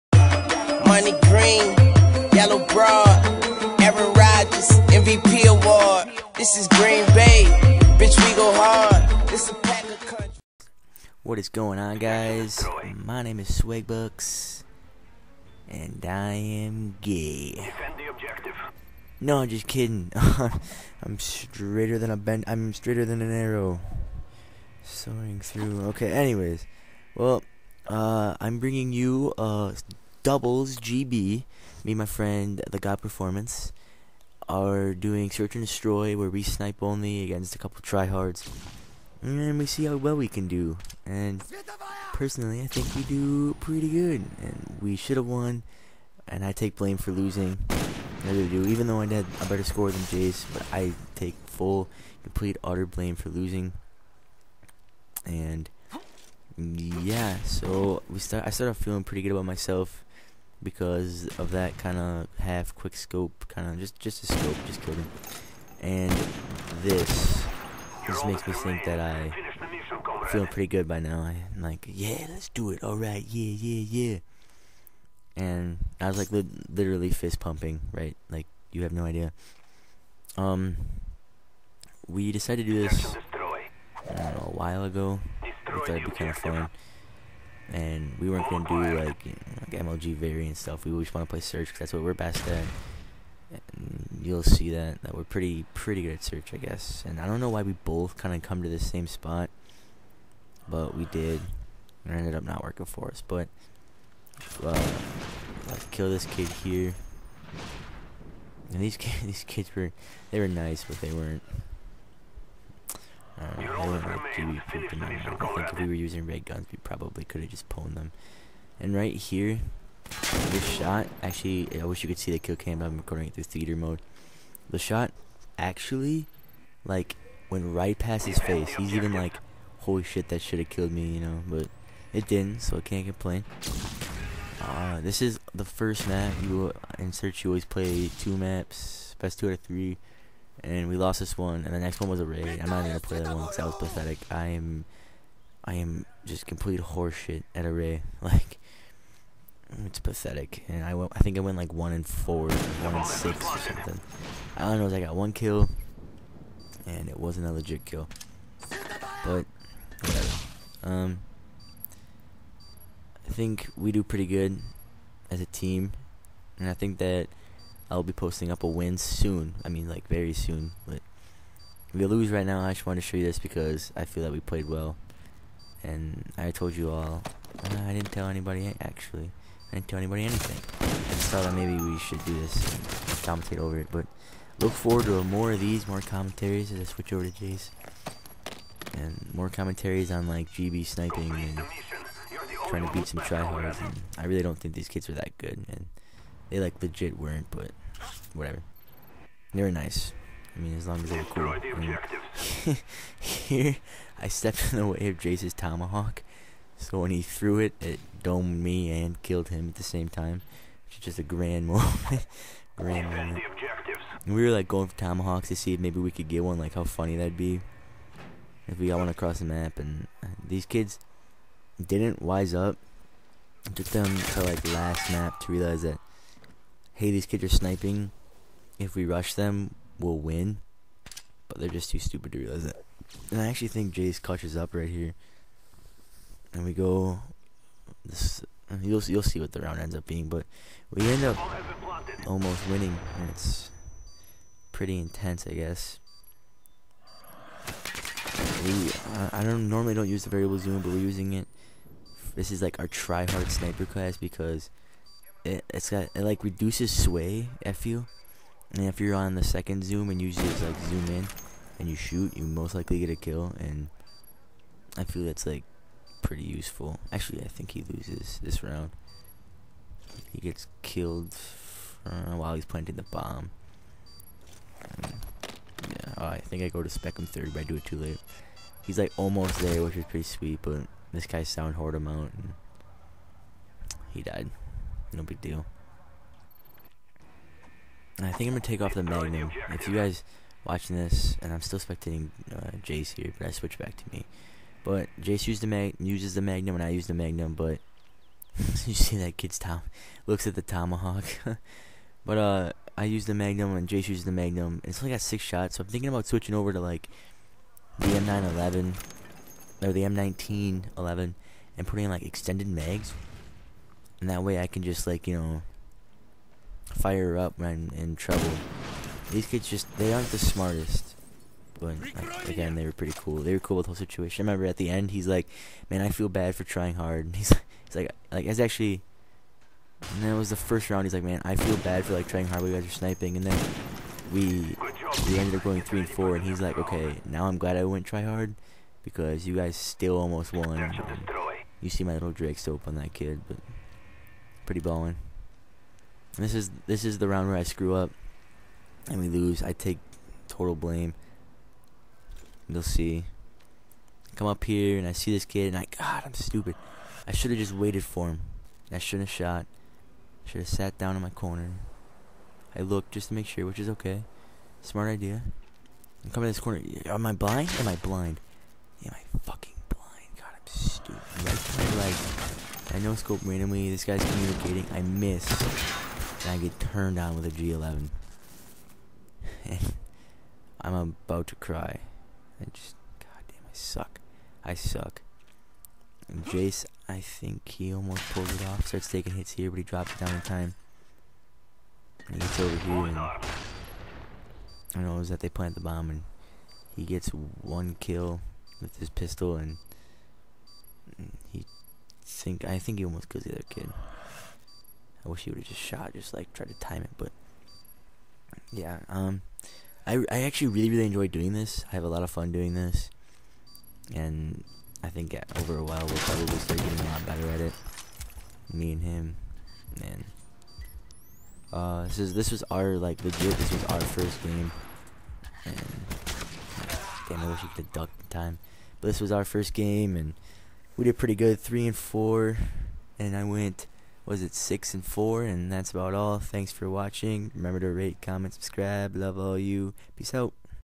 Game Battles 2v2 snipers vs try hards commentary. Episode: 1
yeah we lost but it was close. first one with more to come.